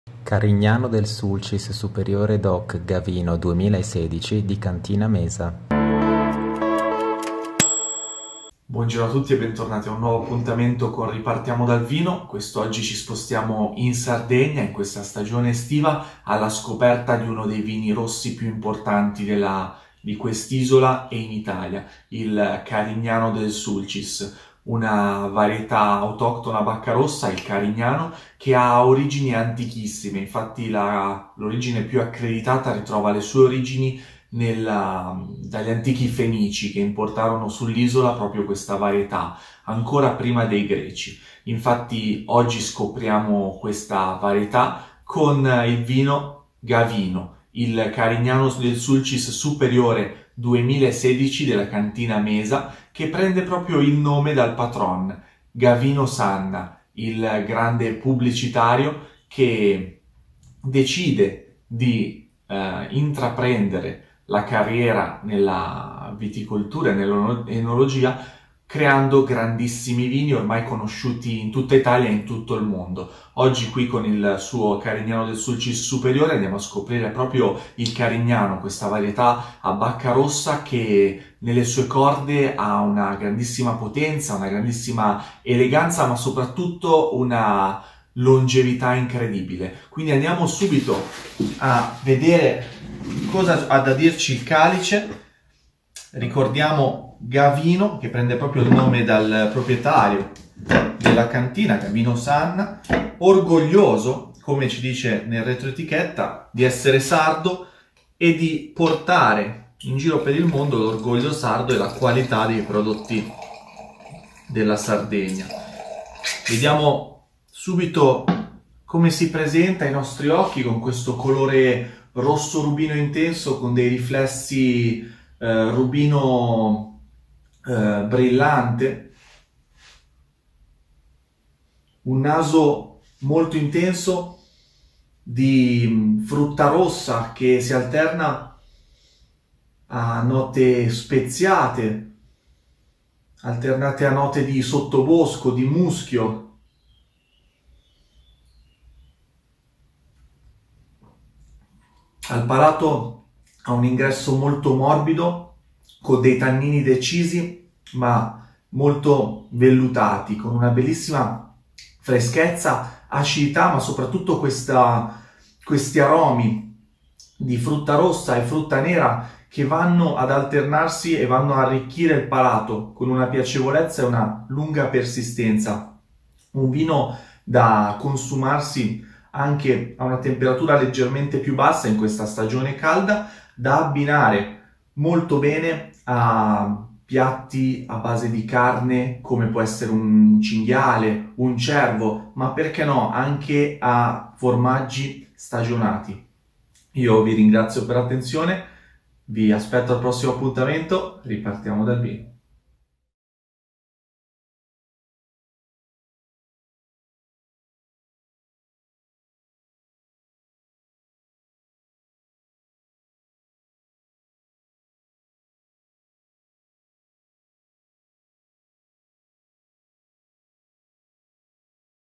Ripartiamo dal vino: “Gavino” 2016 Mesa
Il nome Mesa - che in sardo come in spagnolo significa tavola - rappresenta in sole quattro lettere l’essenza della cantina. Nutrimento, convivio, amore materno, semplicità e profumi. Di terra sarda. Così nasce la cantina Mesa, come una dichiarazione d’amore per la Sardegna, un connubio di bellezza e di bontà, una celebrazione della sua generosità e della sua cultura, attraverso uno dei suoi preziosi tesori: il vino. Così si realizza il sogno di Gavino Sanna, il pubblicitario italiano più famoso e premiato, il sogno di creare qualcosa di straordinario, unico e semplice come la sua terra, per la sua terra. Mesa si trova in terra di Sant’Anna Arresi, sulle pendici di una valle riparata dai venti di maestrale e circondata dal Mar Mediterraneo. Domina la pianura di “medaus” (piccoli borghi rurali, tipici del Sulcis) e l’insenatura del promontorio di Porto Pino. I vigneti di Mesa si estendono per oltre 78 ettari, di cui 66 di proprietà. Le viti sono allevate a cordone speronato, Guyot e ad alberello, a seconda delle condizioni locali e dell’esposizione.

Vino degustato: Carignano del Sulcis Superiore Doc “Gavino” 2016 Mesa

Il colore è rosso rubino intenso, con leggeri riflessi granati. Profumo ricco per complessità e sfumature. Avvincente nell’integrazione tra frutta rossa e nera matura e macchia mediterranea, fra le spezie dolci e le note terziarie di tabacco e cioccolato. Al palato è un vino generoso e armonico, con la freschezza a dettare la lunghezza, la trama carnosa a definirne l’ampiezza mentre la profondità è resa percepibile da tannini grintosi e una sotterranea sapidità. Una tridimensionalità gustativa che fa da rampa di lancio al rincorrersi aromatico di note fruttate, balsamiche. Ideale in abbinamento a selvaggina da pelo, cervo in umido, cinghiale in agrodolce, formaggi stagionati.